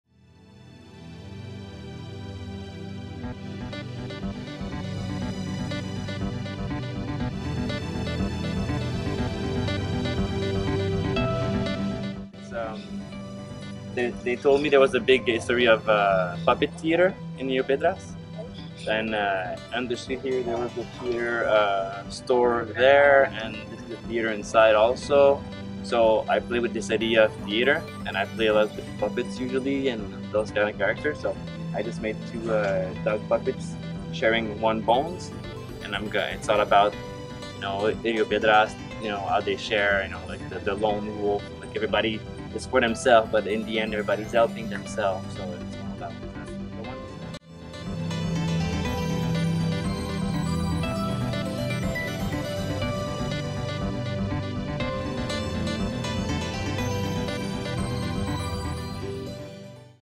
Los muros hablan: Fred Caron
Fred Caron, artista canadiense, busca expresar la unión de dos polos al final de una búsqueda individual.

Entrevista: Michel Estades
Cámara y edición: Giovanni Maldonado